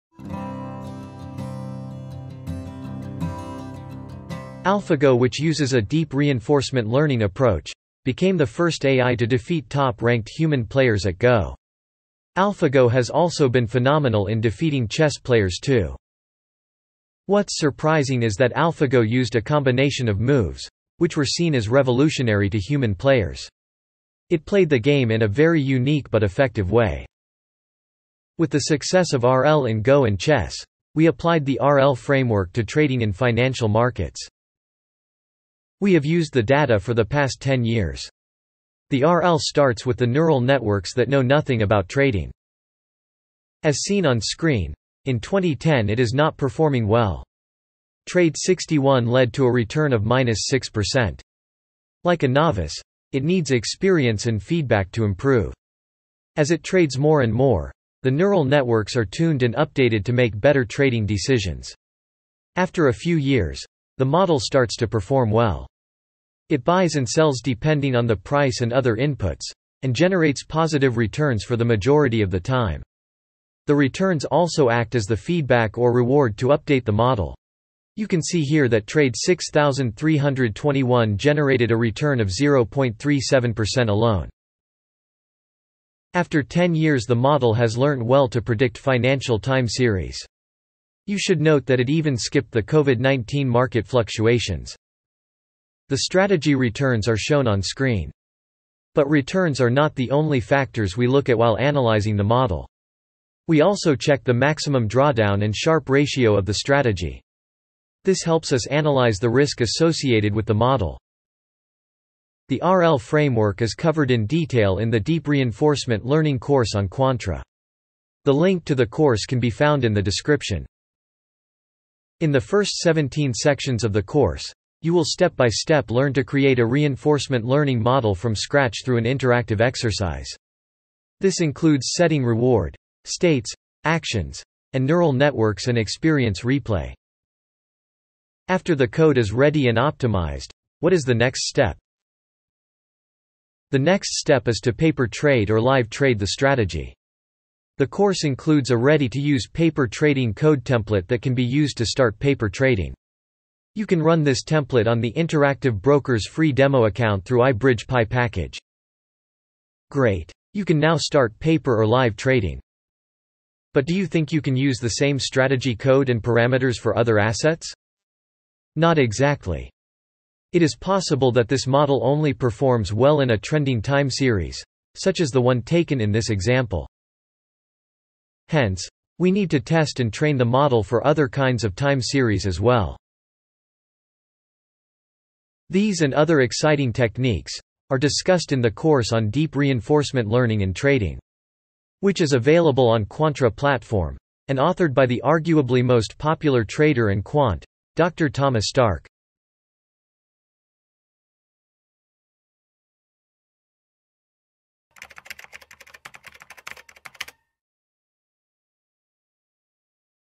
Deep Reinforcement Learning for Trading | Backtesting & Trading | Dr. Thomas Starke | Quantra
This video explains the process of backtesting a deep reinforcement learning on real-life price data. Further, once the backtesting process is completed, you will analyse its performance using Sharpe Ratio and Maximum drawdowns.

Once you are satisfied with the performance parameters, you can use the ready made template to paper trade in live markets and if successful, take it live.